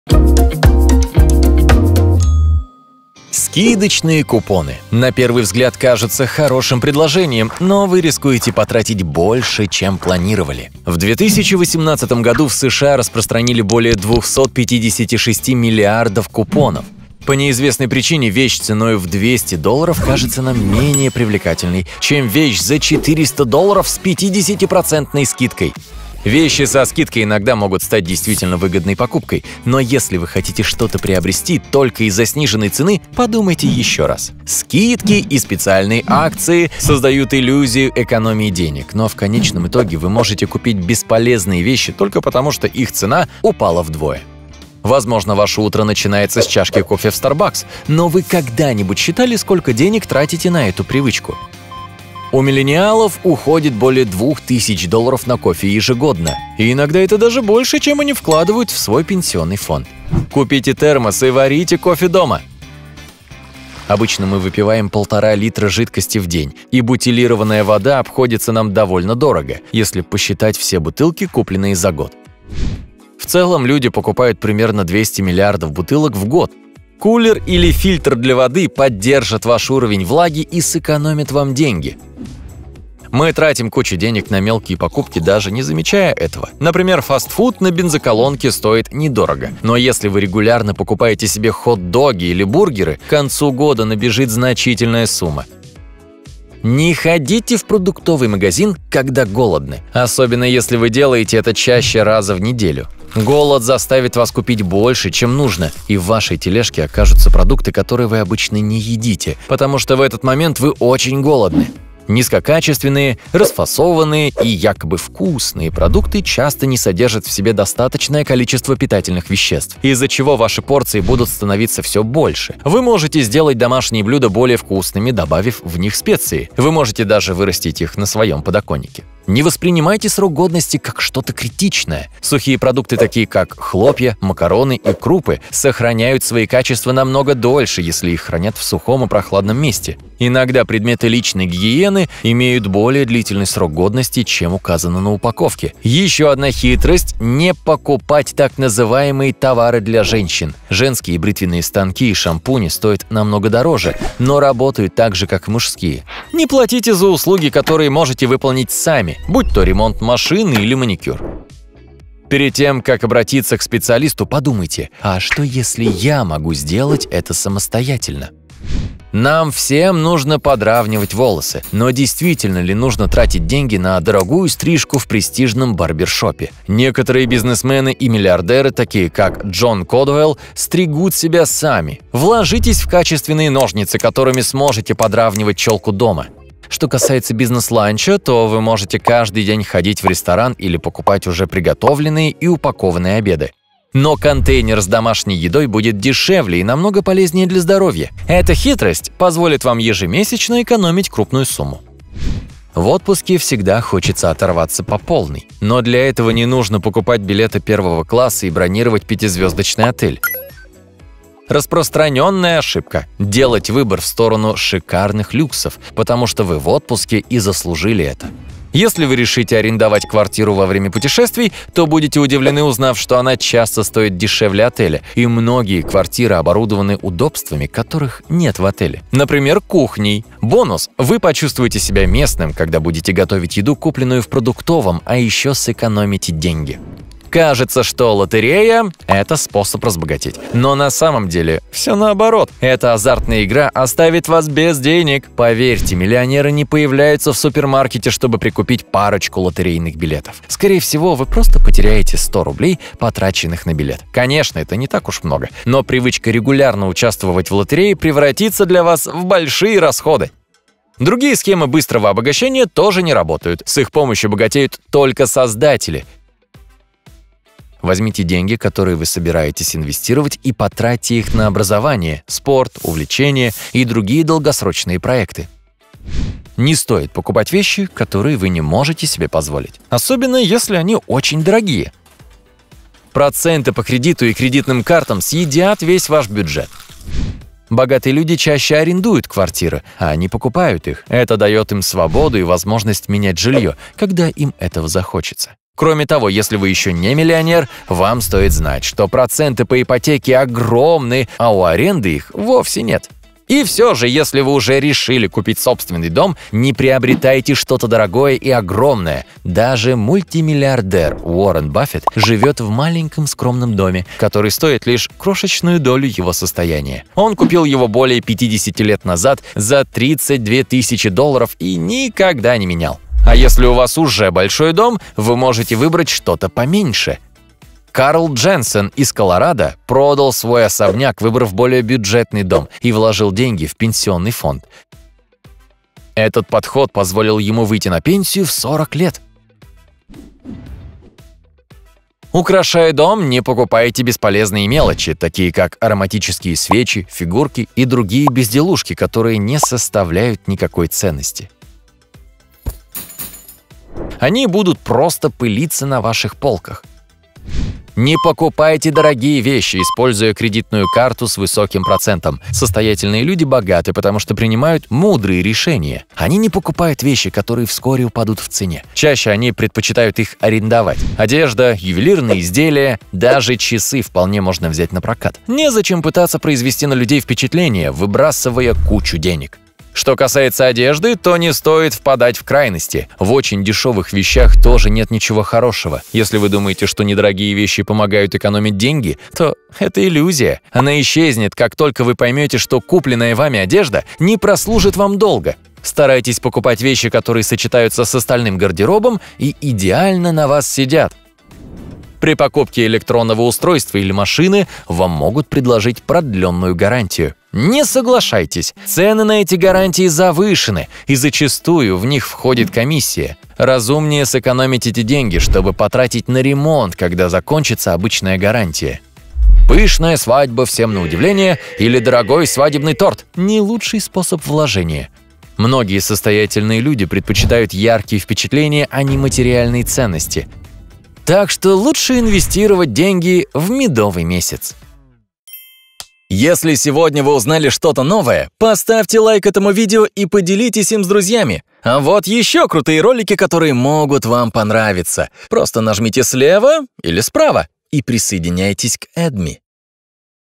Я перестал тратить деньги на эти вещи, чтобы больше не быть бедным
Как быстро накопить деньги? Есть люди, которые умеют экономить и ни в чем себе не отказывать, а есть те, кто тратят деньги направо и налево и становятся банкротами. Если вы больше относитесь к последним, вы все еще можете исправить свою финансовую ситуацию, начав тратить деньги с умом и больше экономить. Не просто так есть поговорка «копейка рубль бережет».

Мы составили список вещей, на которые вам нужно перестать тратить деньги. Мы не предлагаем вам перестать наслаждаться жизнью, просто эти советы помогут вам сохранить деньги в кармане. Например, ваше утро начинается с чашки кофе в Starbucks, но вы когда-нибудь считали, сколько денег тратите на эту привычку? У миллениалов уходит более $ 2 000 на кофе ежегодно!

Тайм-коды:

Вещи со скидкой 0:03

Кофе на вынос 0:44

Бутилированная вода 1:04

Перекусы на ходу 1:22

Низкокачественные вкусные продукты 1:36

Товары для женщин 2:19

То, что вы можете сделать сами 2:36

Отпуск 3:22

Лотерея 3:56

Схемы быстрого обогащения 4:20

Аренда квартиры 4:47

Собственный дом 5:03

Кредитные карты 5:58

Одежда 6:19

Пышная свадьба 6:59